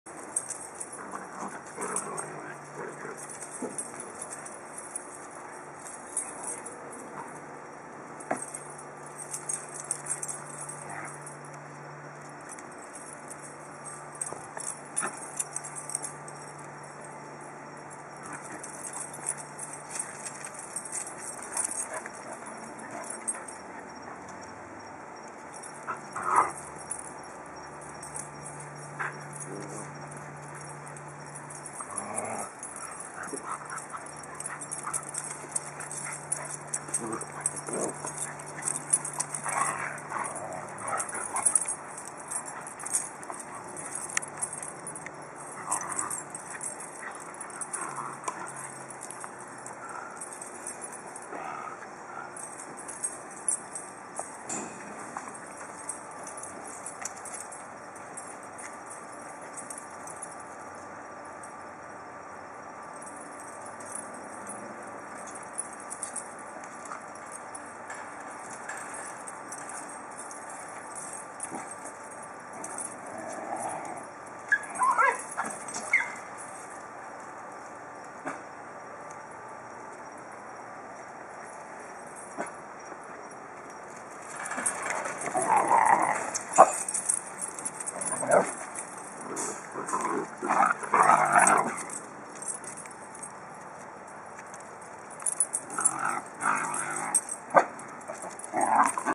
Super Basenjis at play
The latest batch of pups turned over to rescue by a hoarder play with the Wimauma basenji adults and former pups.

This kind of play is great - lots of exercise. The stones and pavers everywhere are covering more destructive 'play'... The Wimauma pack had lots of serious diggers.